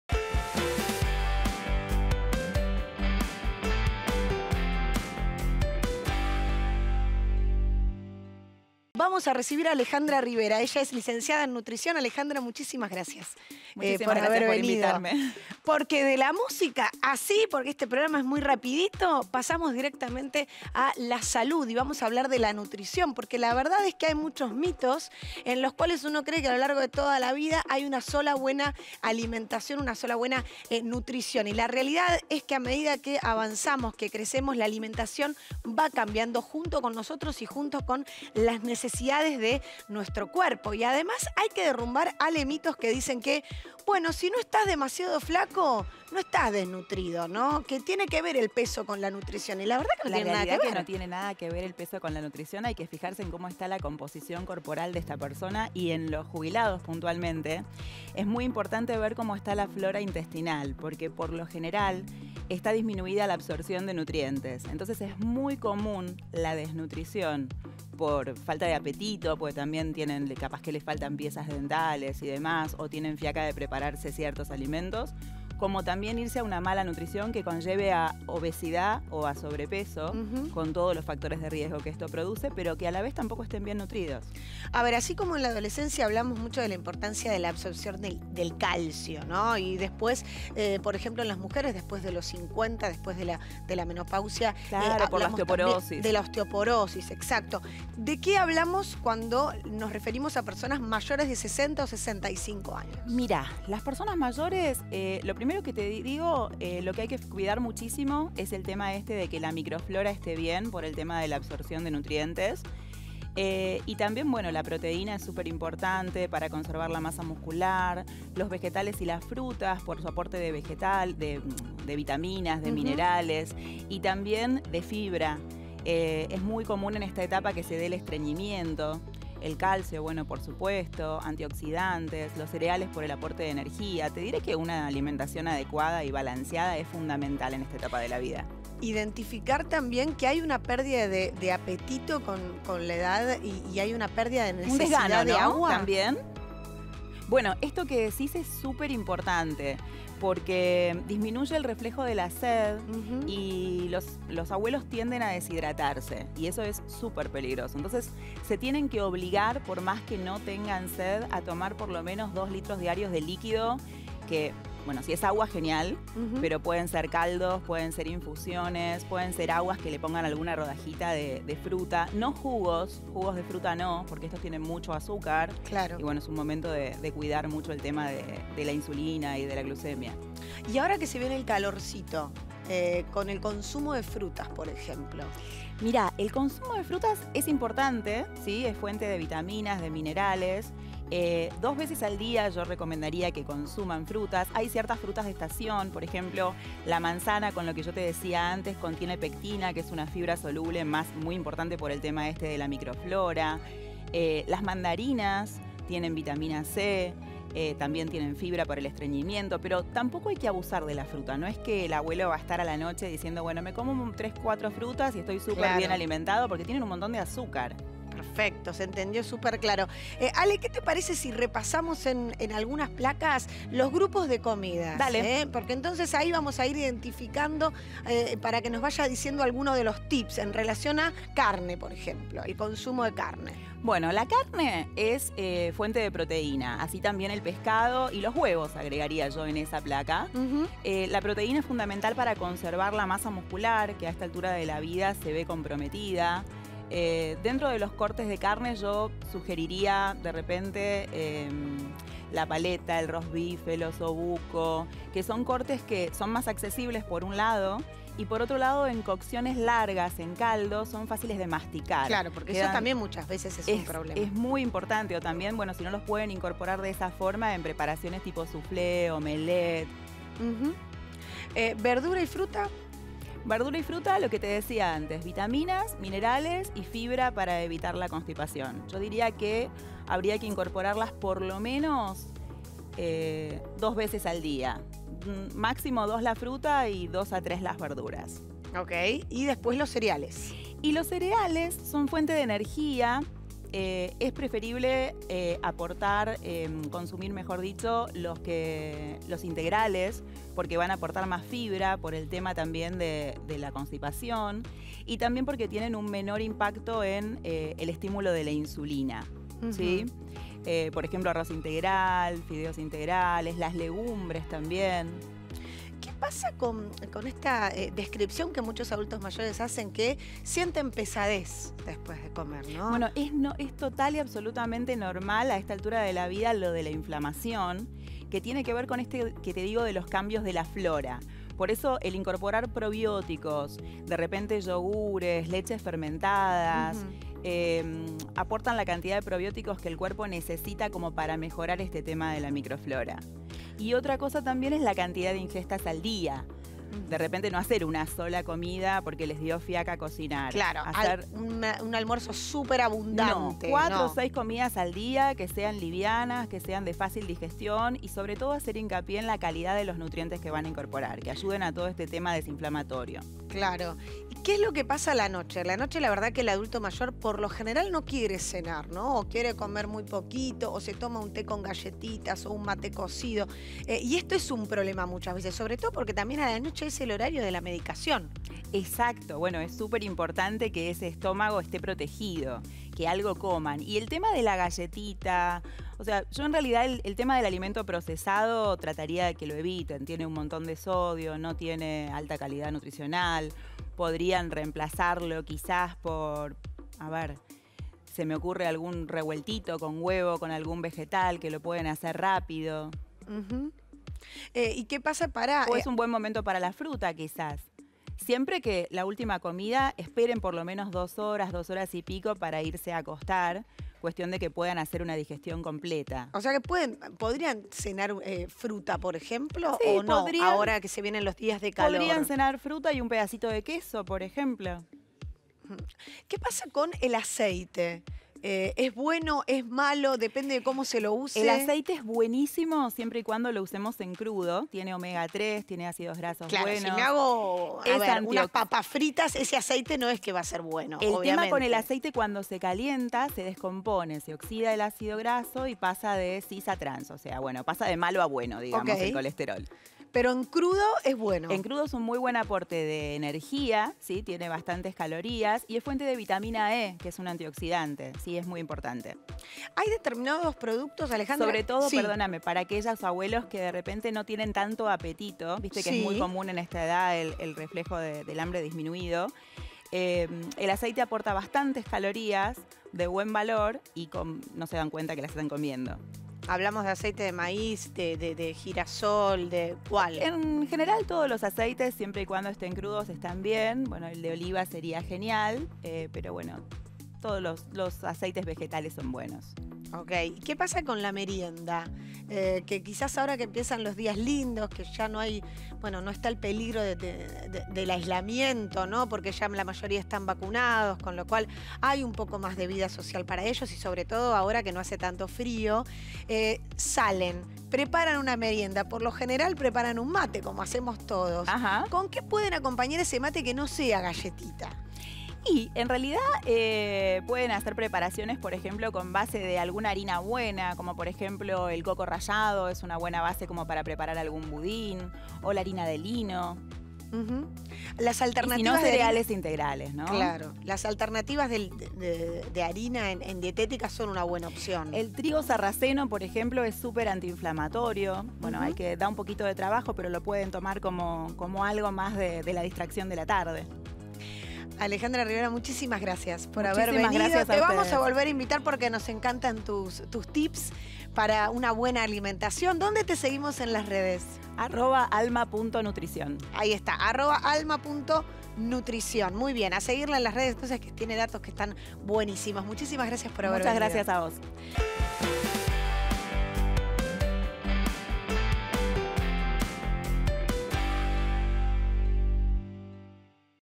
Comer bien: qué tener en cuenta para una buena nutrición en la tercera edad
Para ahondar en esta cuestión, consultamos con un especialista en nutrición. "En jubilados puntualmente, es muy importante ver cómo está la flora intestinal", precisó.